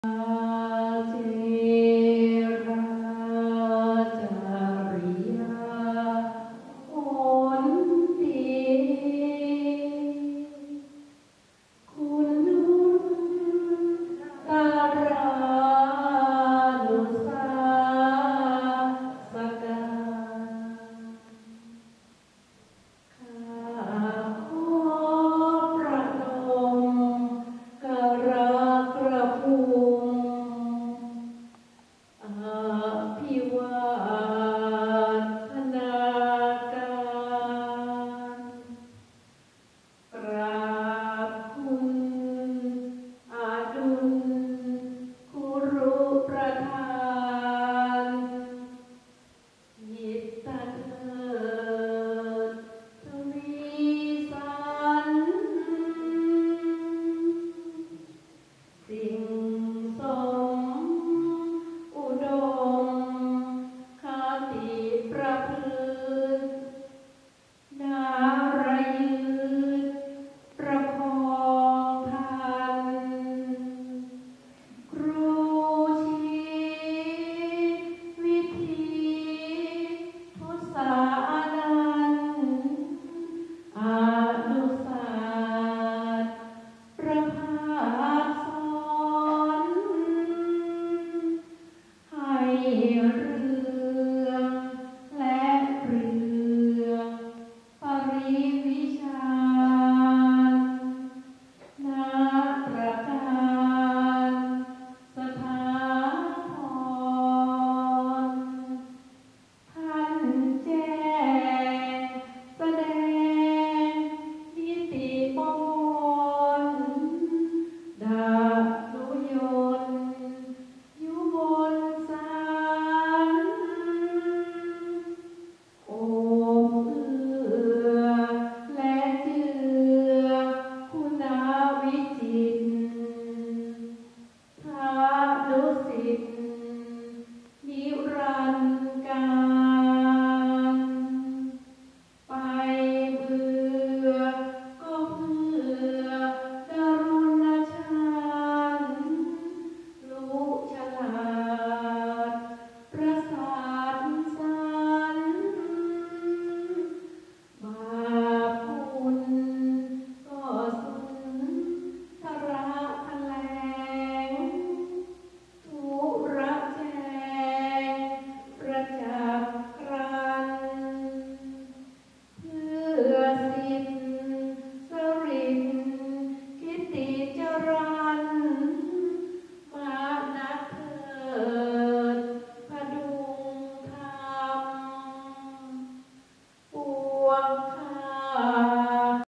พิธีบูชาบูรพาจารย์..วันครูแห่งชาติ จ.เชียงราย ปี 2566
พิธีบูชาบูรพาจารย์..วันครูแห่งชาติ จ.เชียงราย ปี 2566 ณ สหกรณ์ออมทรัพย์ครูจังหวัดเชียงราย..เมื่อวันที่ 16 มค.2566